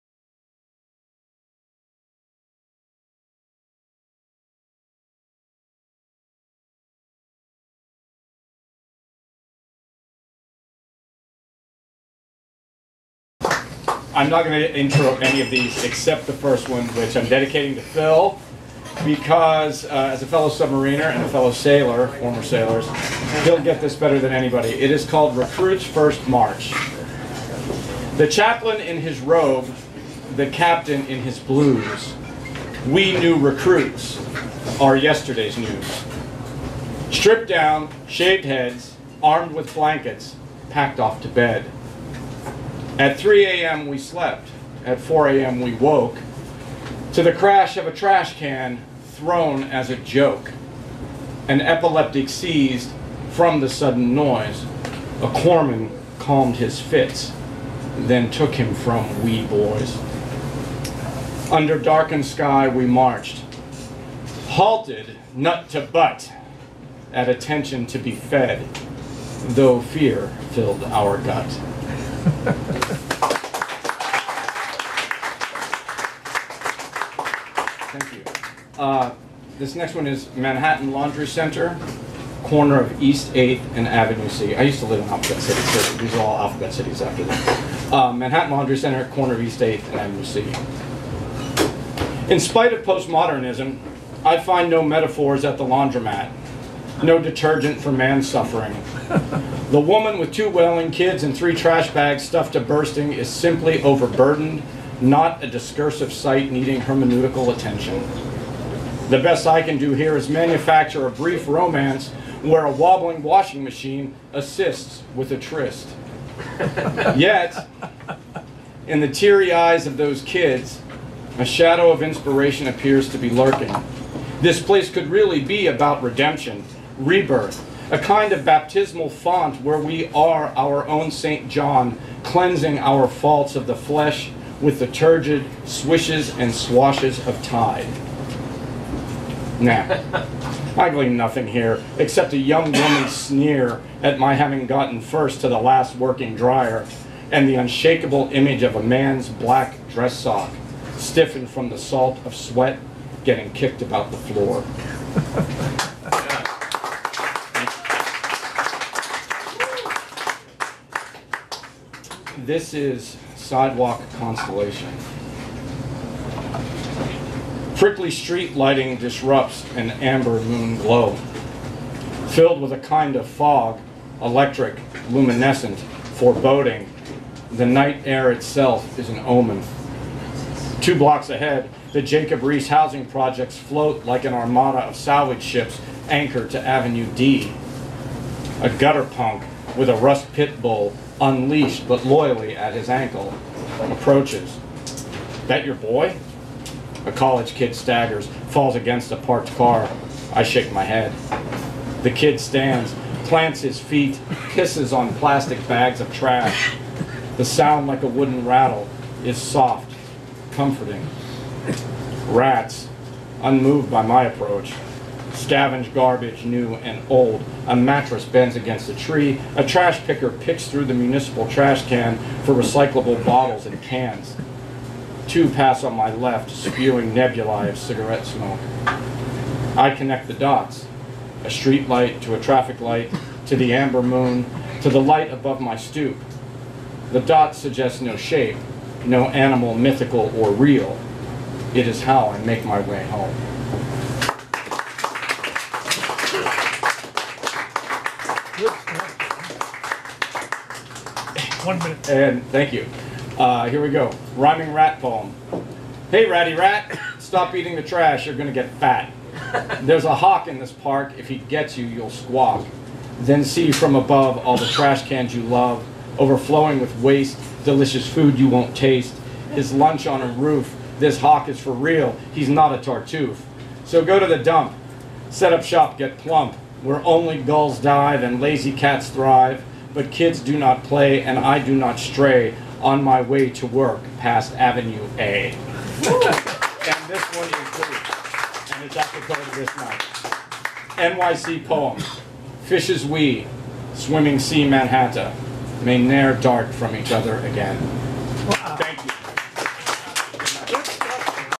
"Open Mic Gypsy" John Reoli for Rimes of The Ancient Mariner August 26, 2015
"Open Mic Gypsy" John Reoli for Rimes of The Ancient Mariner August 26, 2015 @ Three of Cups Lounge.